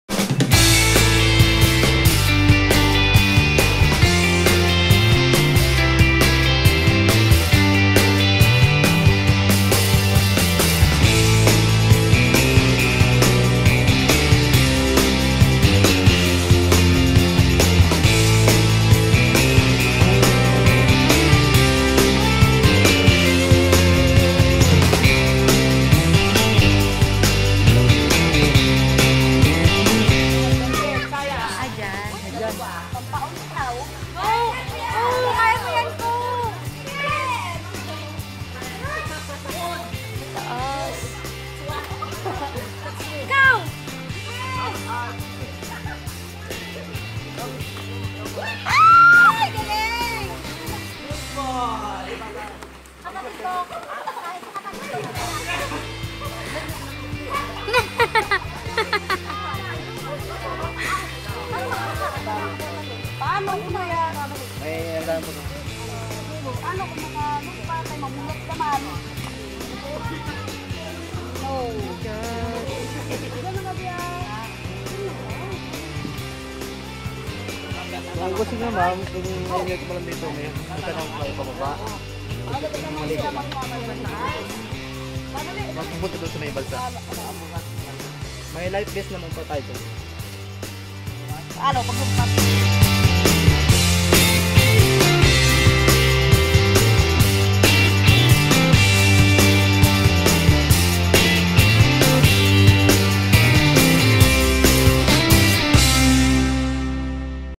What to Explore in Carabao Island: Cathedral Cliff Diving
My first cliff diving experience was nerve racking but it was absolutely amazing!😊

Video and pictures were taken summer last year (April 2018)